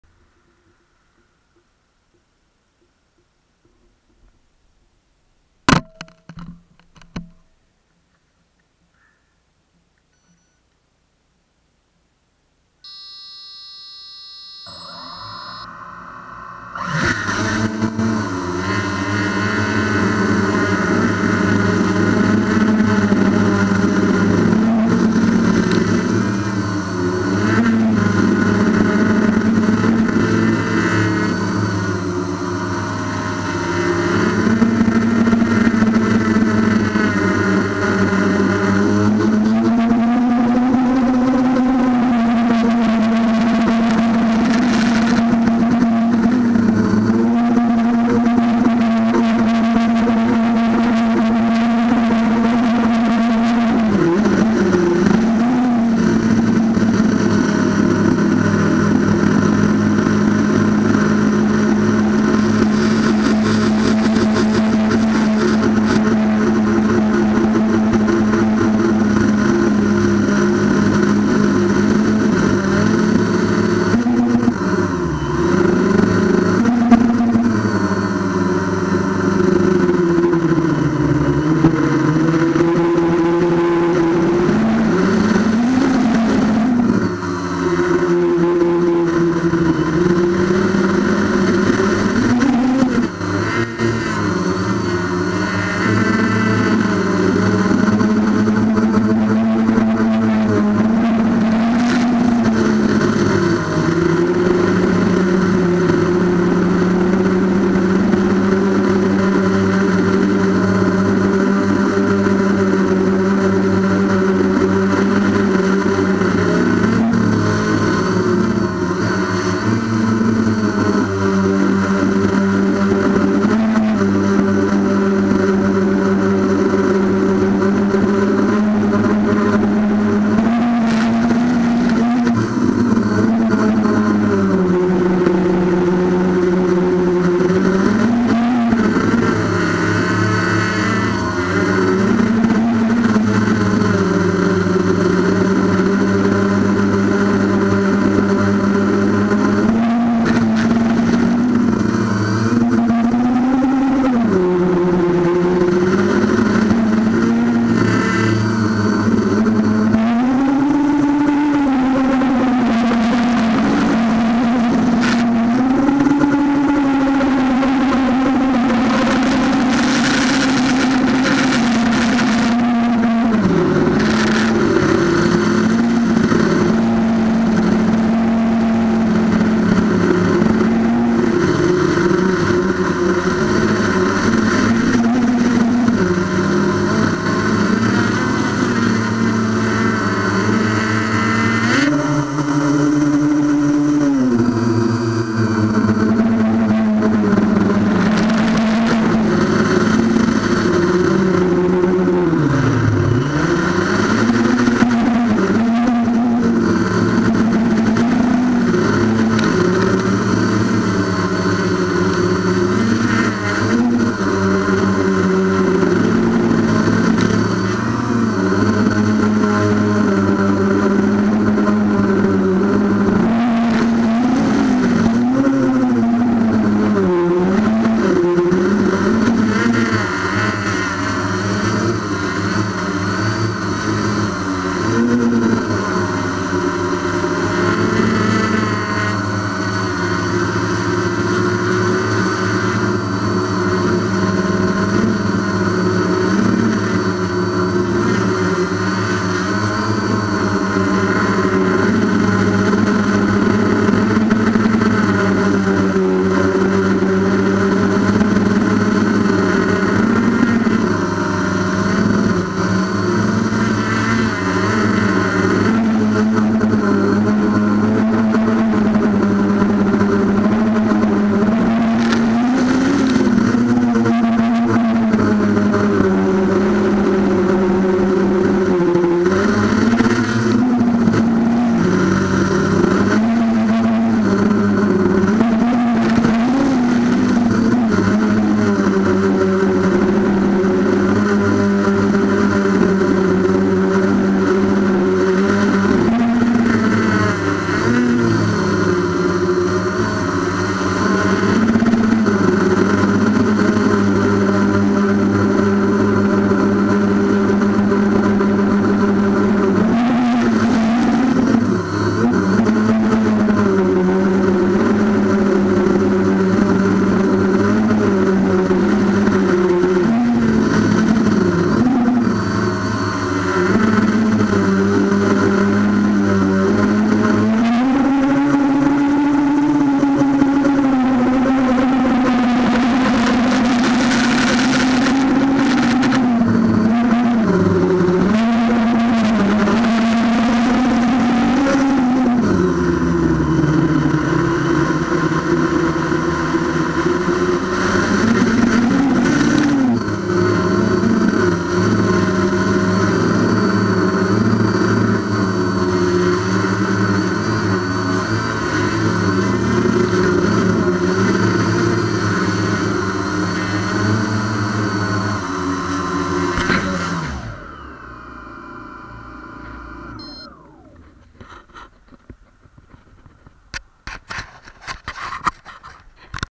Traxxas aton with sports cam
Flying up and down side walk quickly with traxxas aton quadcopter